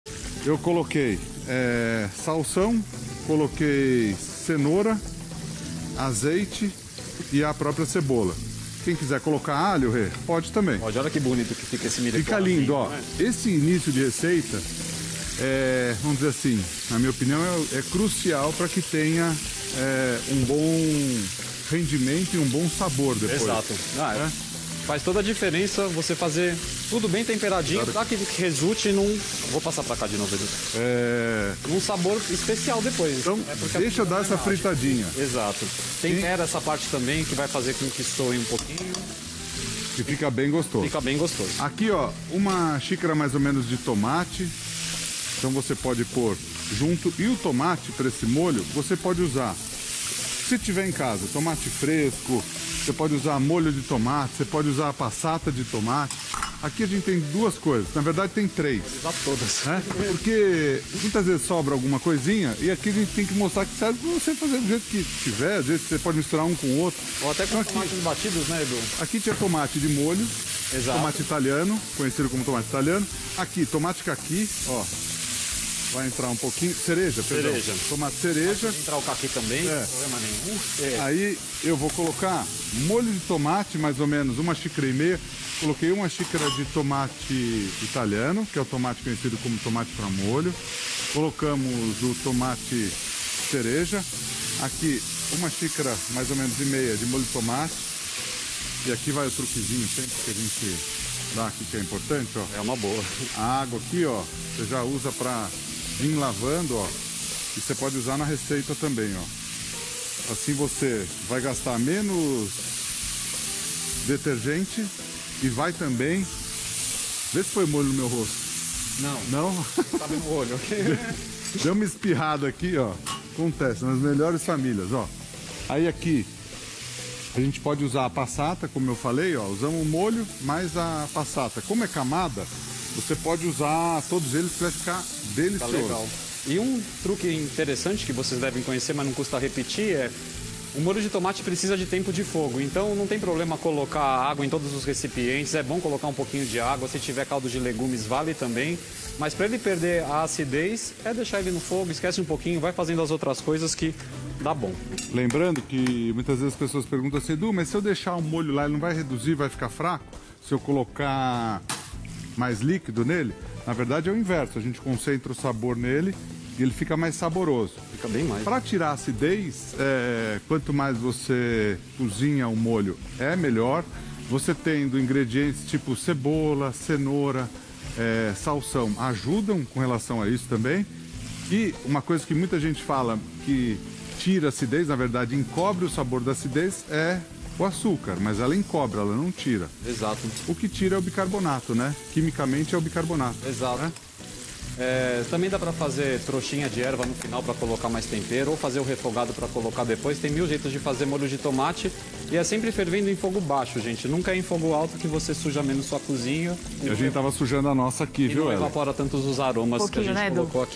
Como reduzir a acidez do molho de tomate? | Band Receitas
Chefs Edu Guedes e Renato Pires explicam como fazer o molho de tomate perfeito: cremoso, com sabor e acidez equilibrada. Spoiler: não precisa colocar açúcar!